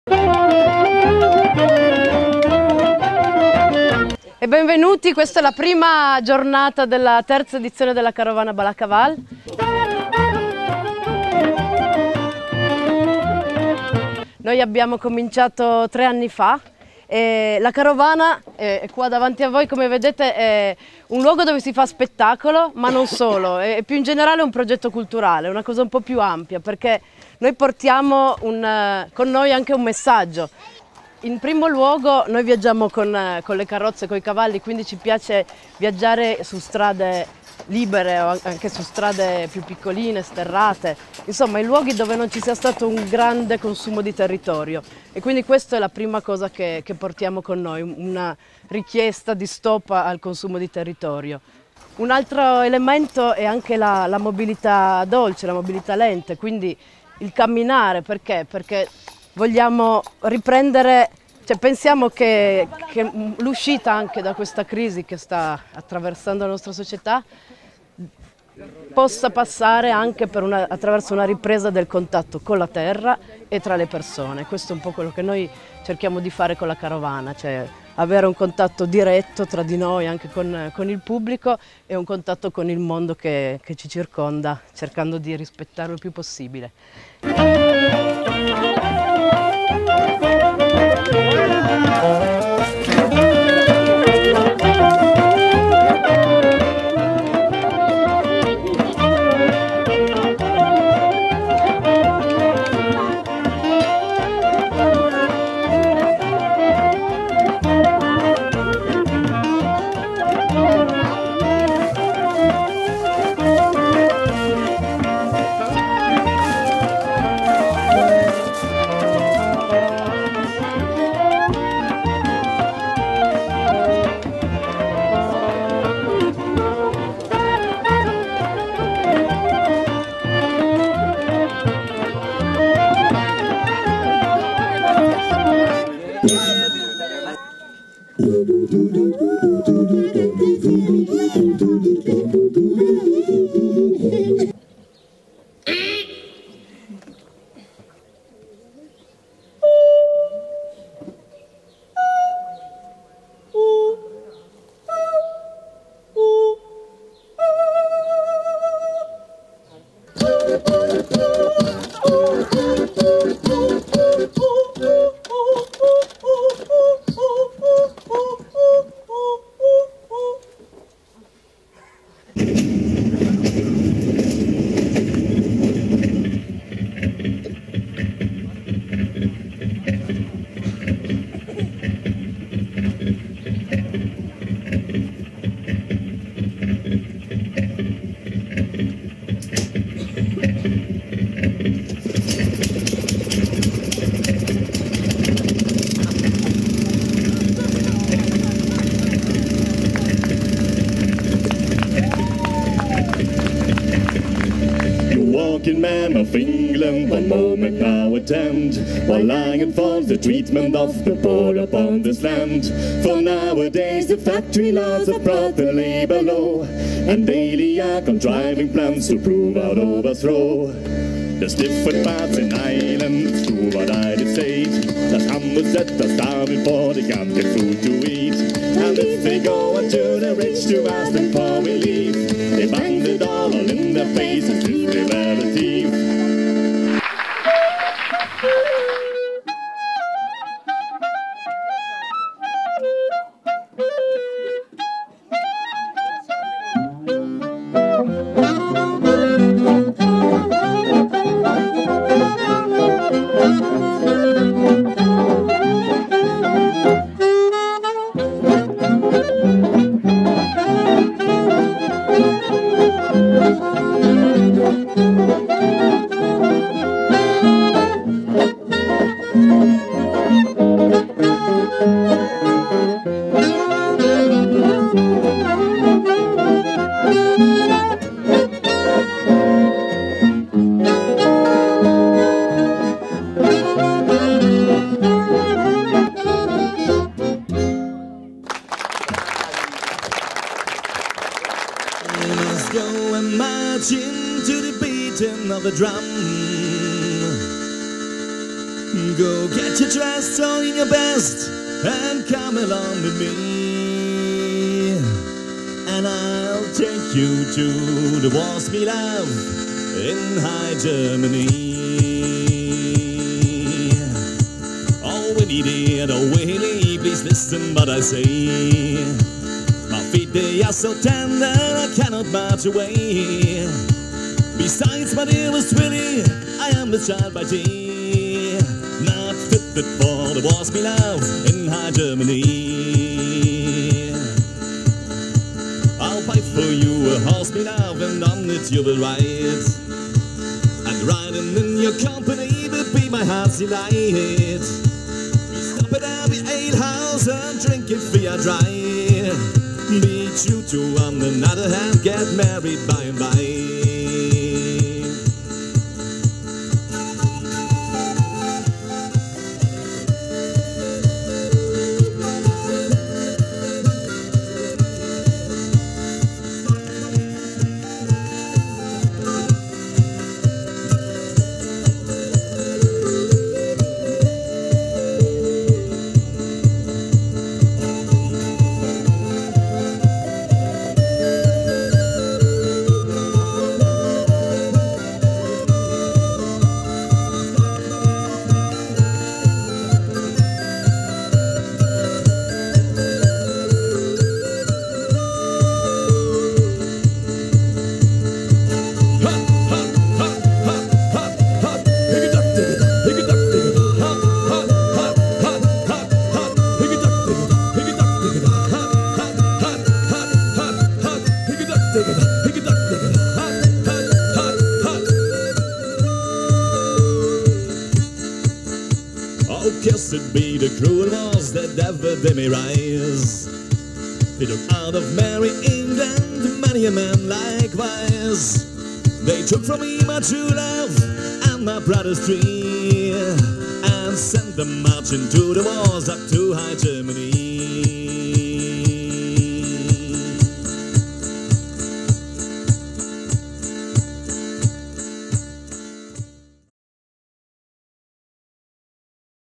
Carovana depart 2013 (pierre valet) 7'29
festa di partenza 2013 alla Cascina del Bric podio di Bene Vagienna (CN)